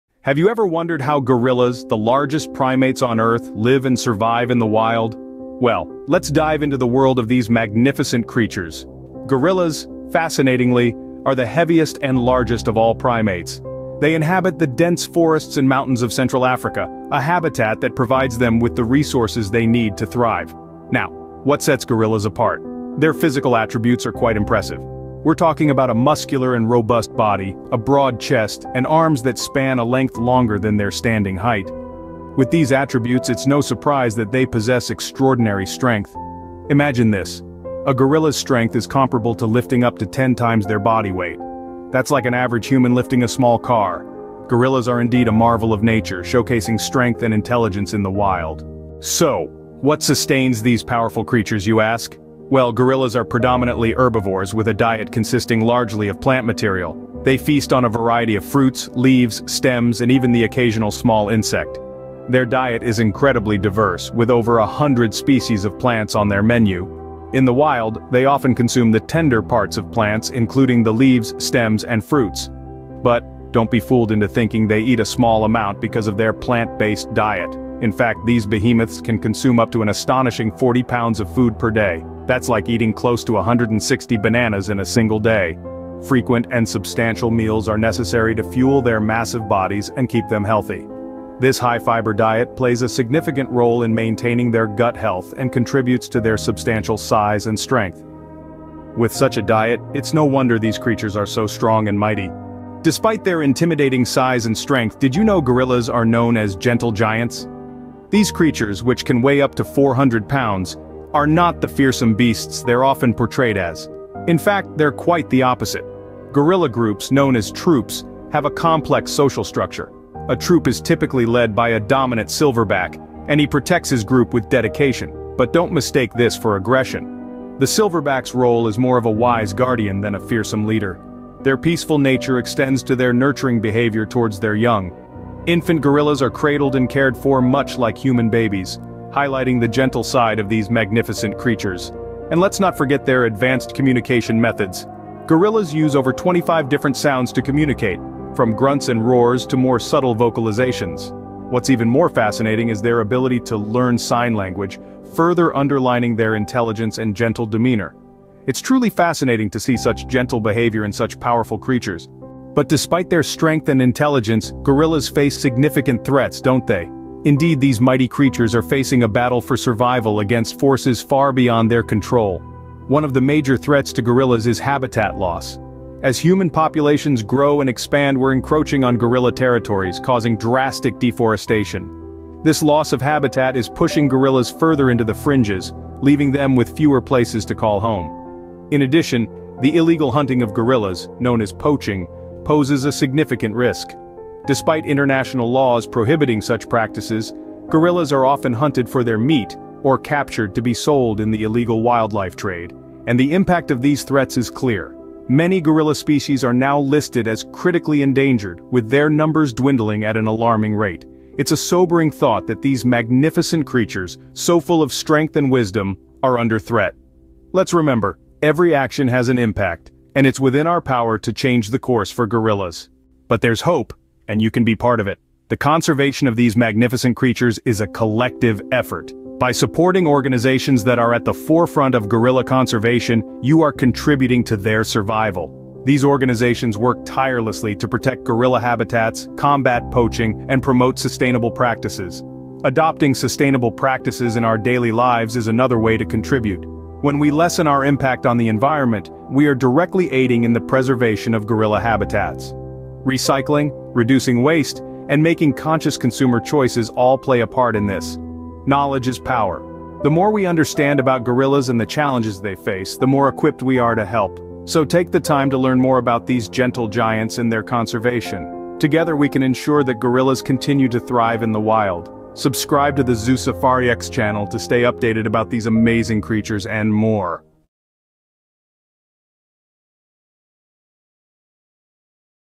Gorilla The Gentle Giants of ZooSafariX
Discover the captivating world of gorillas, the gentle giants of ZooSafariX. In this heartwarming documentary, we take you on a journey through their lives, their remarkable behaviors, and the dedicated efforts to protect and preserve this endangered species. Join us in exploring the beauty and intelligence of these majestic primates in 'Gorilla: The Gentle Giants of ZooSafariX,' where we uncover their incredible story and their important role in wildlife conservation